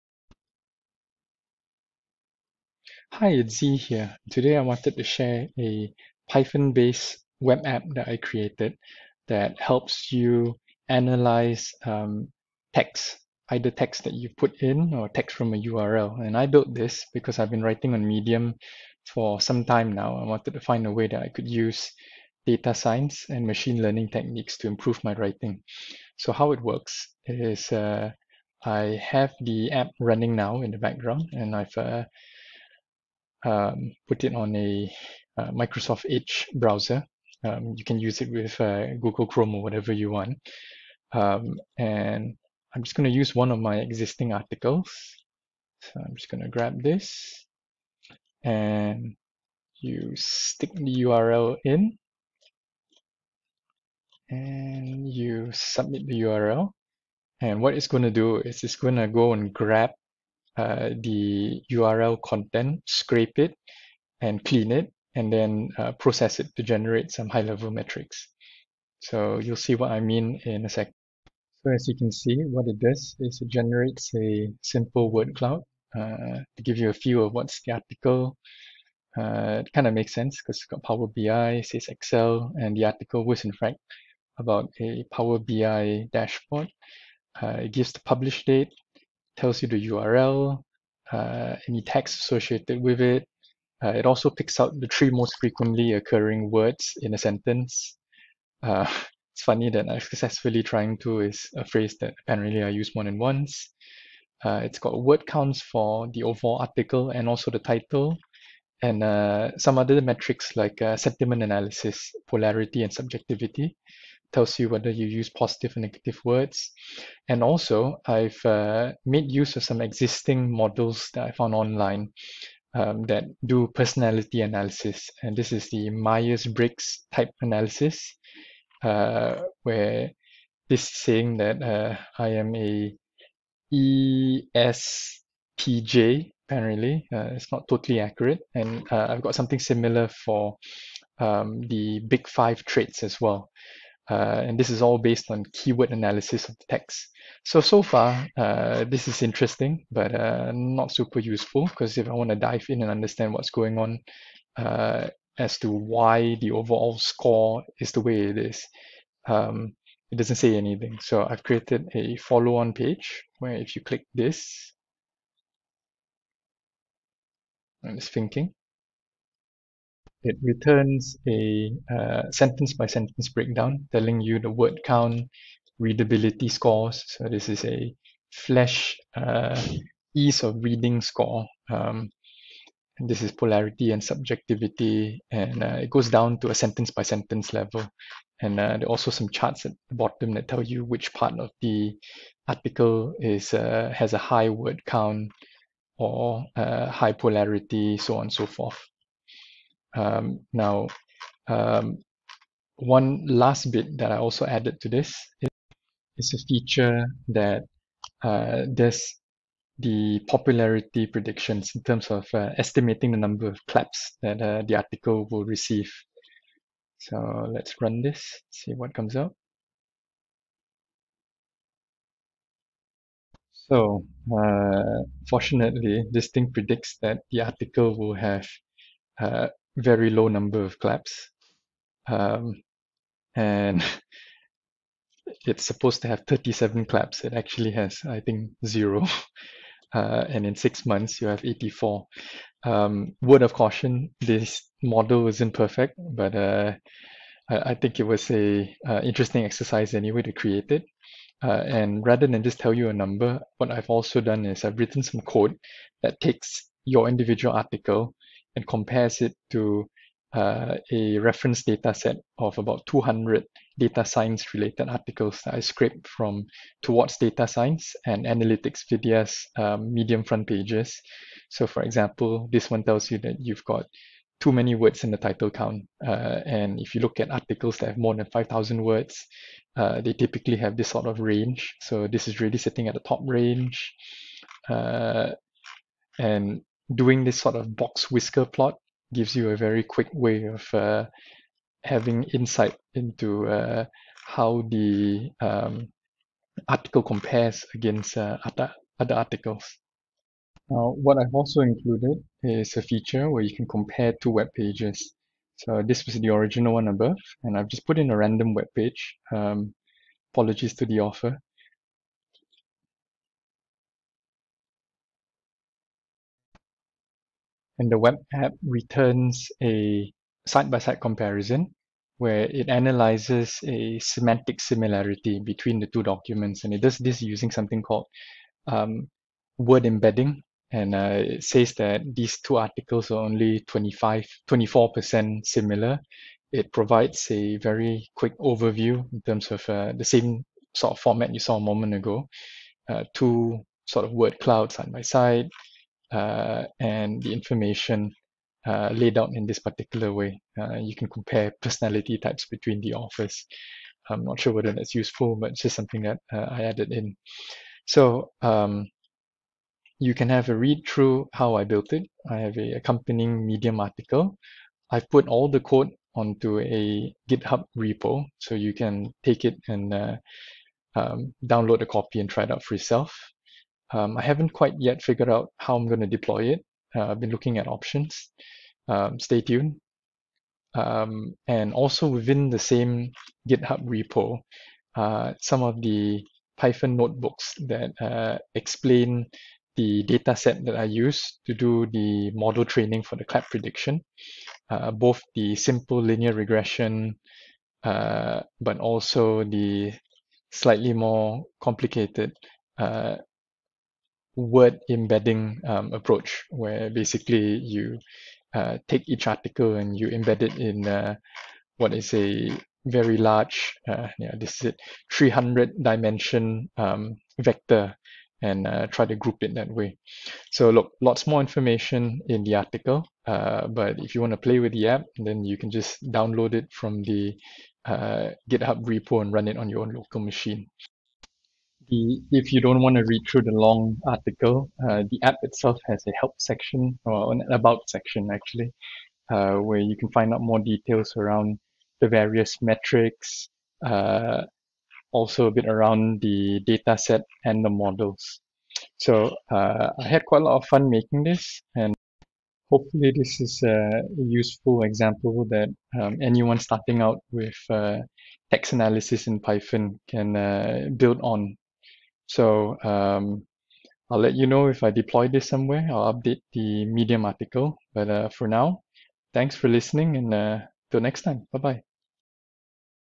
Building A Text Analysis Web App With Python Flask
This is a demo of a simple text analysis web app I built.